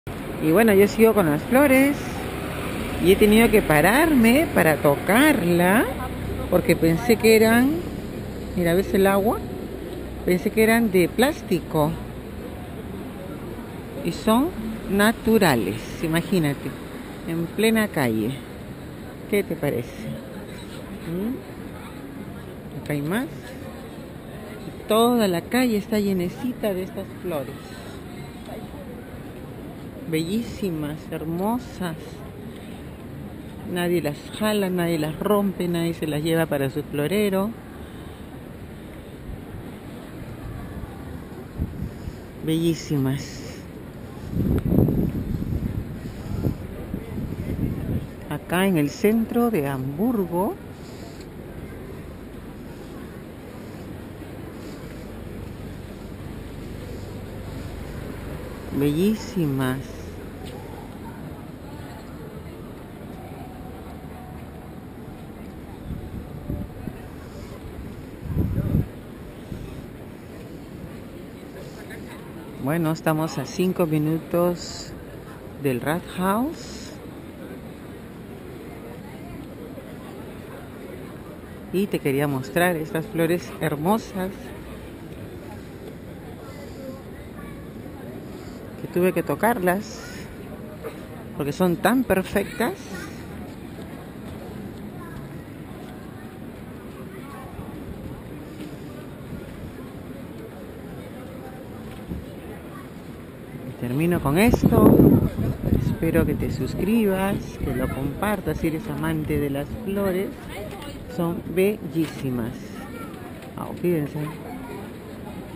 Macetas en la calle realmente hermosas y nadie las toca
Esto es cultura, nadie toca lo que es ajeno, lo respetan al 100% comparte para enseñar a no tocar las cosas que no son nuestras